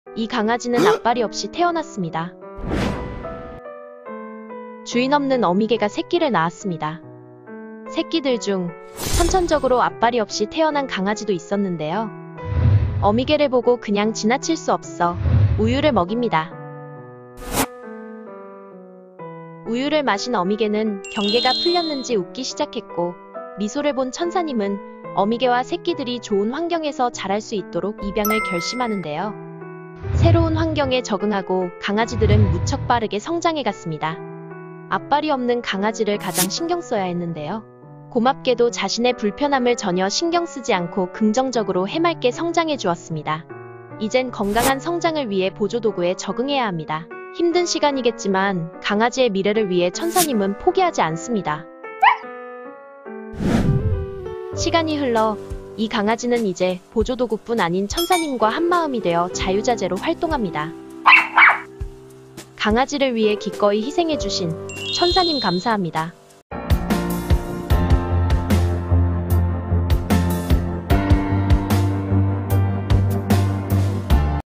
앞발없는 강아지가 행복한 이유
새끼를 낳은 어미 개를 발견한 천사님은 모두 데려와 행복한 미래를 선물했습니다